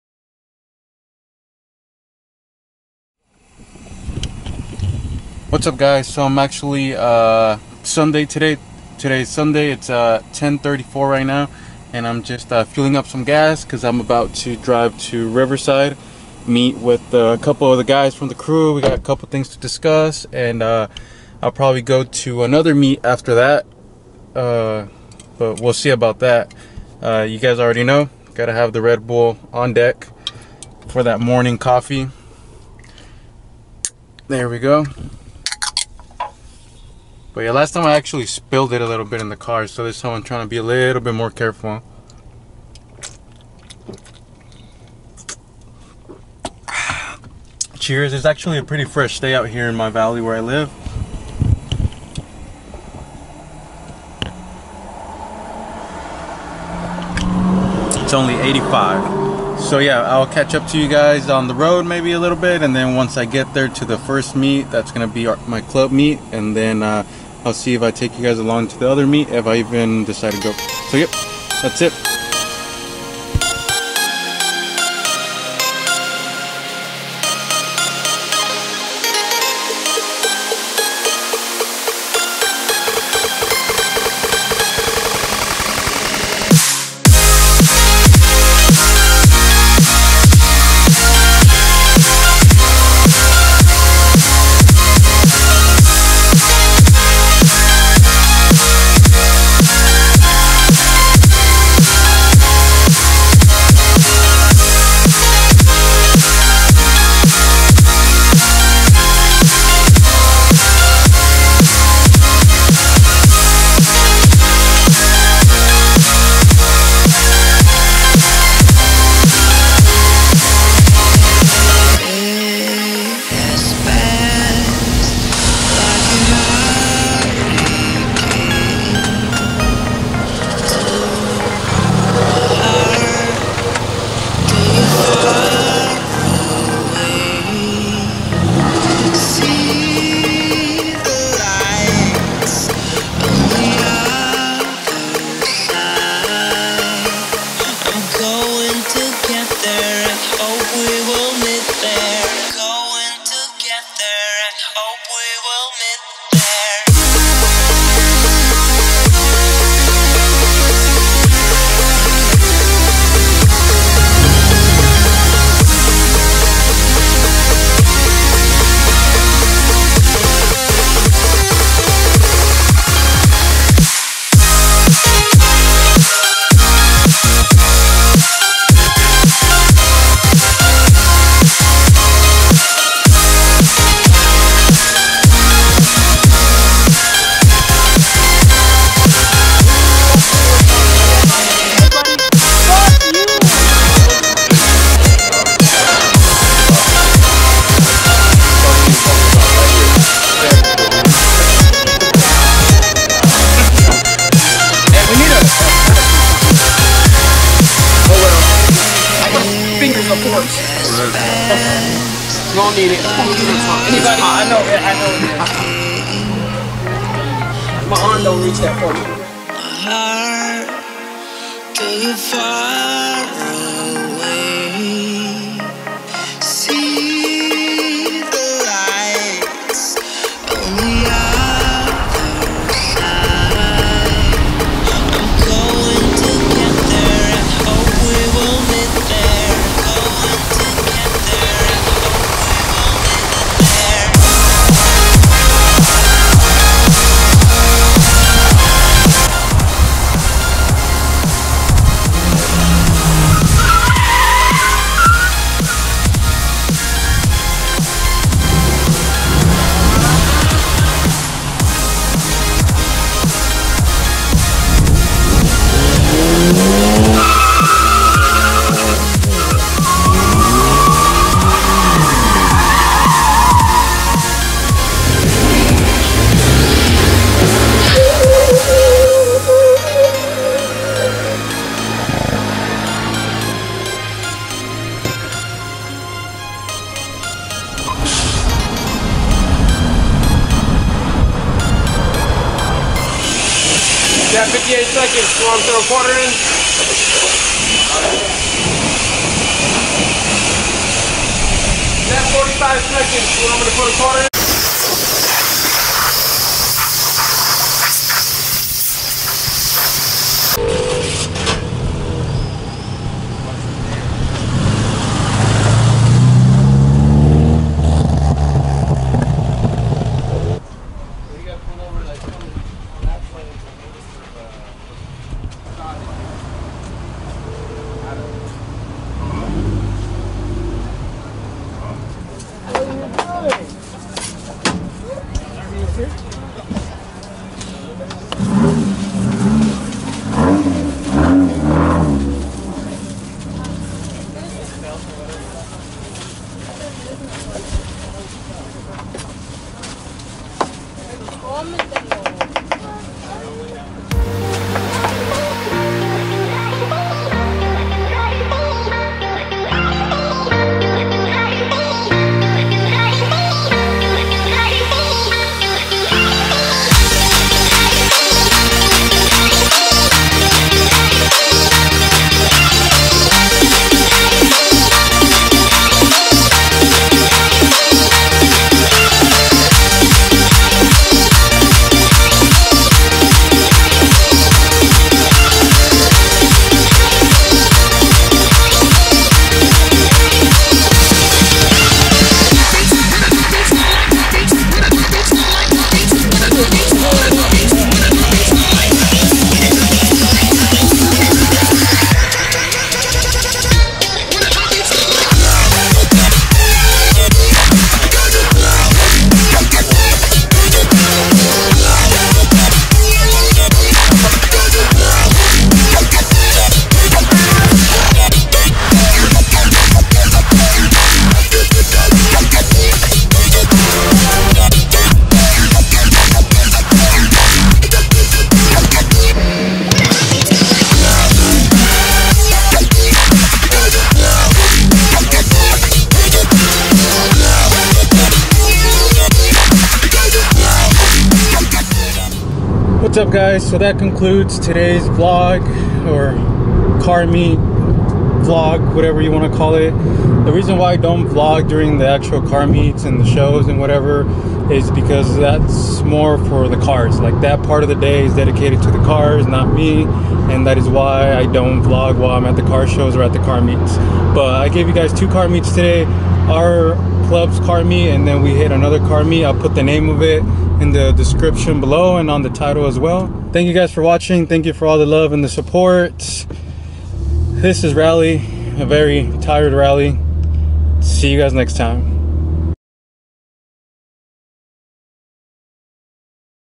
Jamacor Meet/UnitedVelosters Meet.
Hey guys hope you enjoy this video , today we had a UnitedVelosters meet and we also attended the Jamacor meet.. Hope you guys enjoy the cars and our family... Remember at 300subs ill be giving away my rally armor mudflaps so stayed tuned for that!